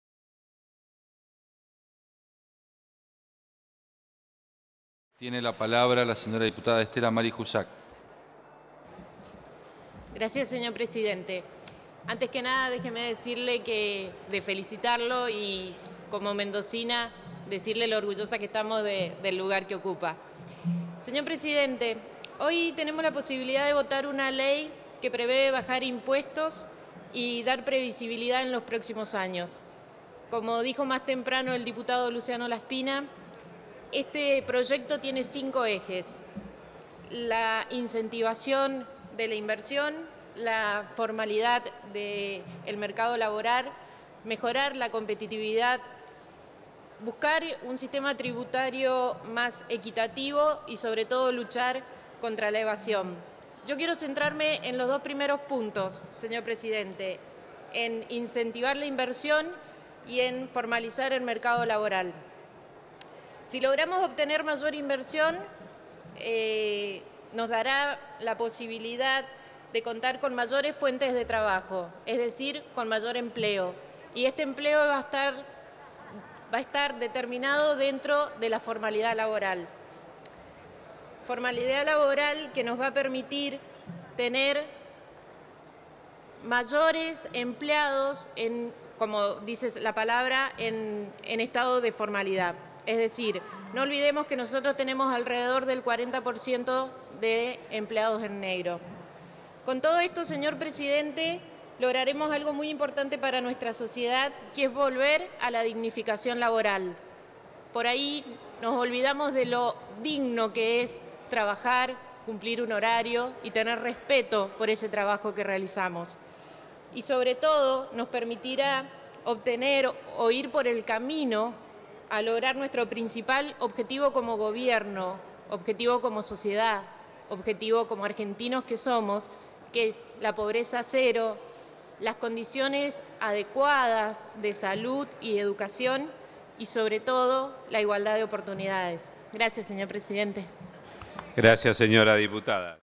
Diputado Huczak Stella Maris - Sesión 19-12-2017 - PL
Continuación 2° Sesión Especial - 24° Reunión

Expte. 20-PE-17) Reforma del Sistema Tributario Argentino. OD 4 Expte. 17-PE-17) Revalúo Impositivo y Contable. Modificación de la ley de Impuesto a las Ganancias. OD 4 Expte 151-S-17) Autorización al señor Presidente de la Nación para ausentarse del país durante el año 2018 cuando razones de gobierno así lo...